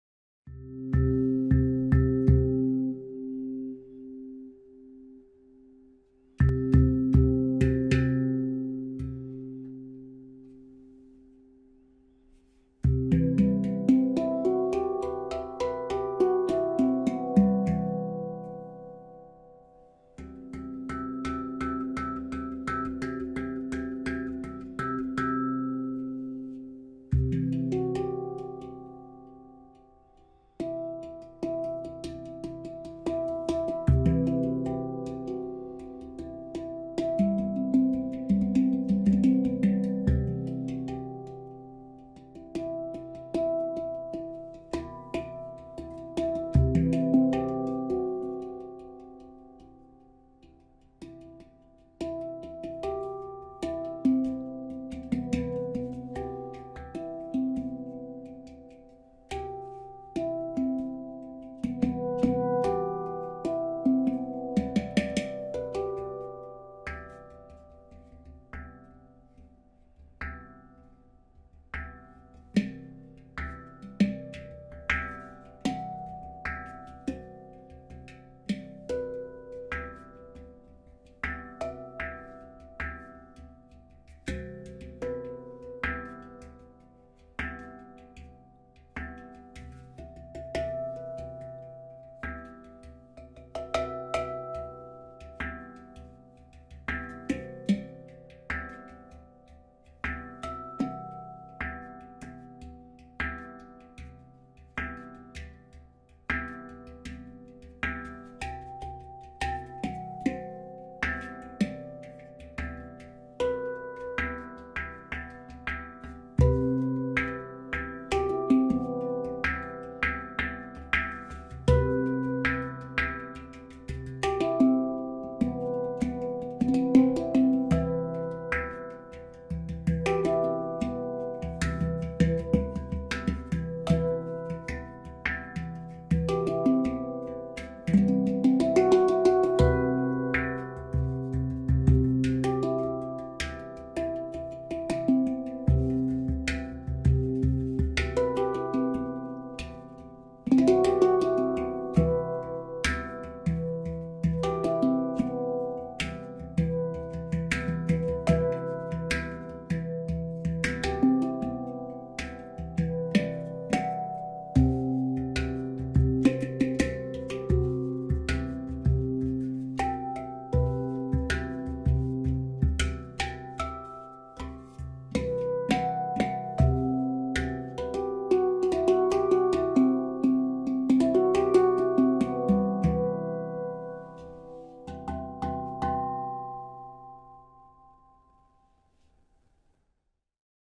Celestial Sound C Athena 10 Handpan (Aegean)
HIGHLY recommend to listen with headphones or a high quality audio system!

C Athena 10
C  /  E G B C E F# G B E

Made on AISI 430 Stainless Steel
Shoulder tuned to E6 and F#6

This is a standard scale offered by Celestial Sound in Australia. Also commonly known as the Aegean scale, we've named it after the Aegean Goddess Athena, for it's wisdom and originality.

In C Lydian, this scale will also play well with E Minor, G Major, A Dorian, D Mixolydian and B Phrygian.